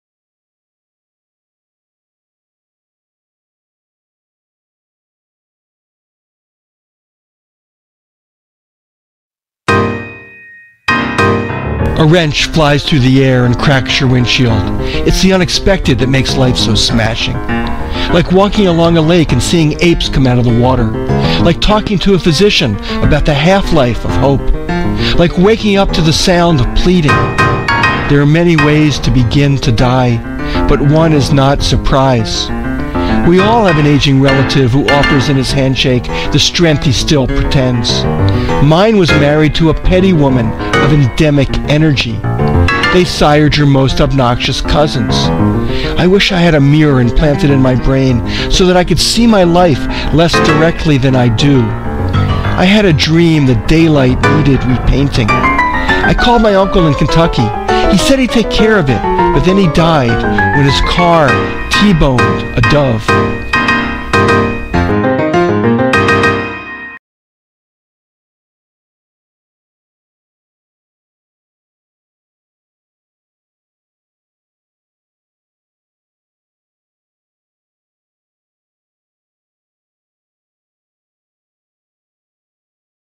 Videopoem: "Whiplash Marriage" by Bill Yarrow and Ray Fahrner
Video Remix of "Whiplash Marriage."
Poem by Bill Yarrow.
Reading and Video by Bill Yarrow.

WHIPLASH MARRIAGE

A wrench flies through the air and cracks your
windshield. It's the unexpected that makes life so
smashing. Like walking along a lake and seeing
apes come out of the water. Like talking to a physician
about the half-life of hope. Like waking up to the sound
of pleading. There are many ways to begin to die
but one is not surprise. We all have an ageing relative
who offers in his handshake the strength he still pretends.
Mine was married to a petty woman of endemic energy.
They sired your most obnoxious cousins. I wish I had
a mirror implanted in my brain so that I could see my life
less directly than I do. I had a dream the daylight needed
repainting. I called my uncle in Kentucky. He said he'd take
care of it, but then he died when his car T-boned a dove.

This poem was published in Poetry International. It appears in Pointed Sentences (BlazeVOX, 2012).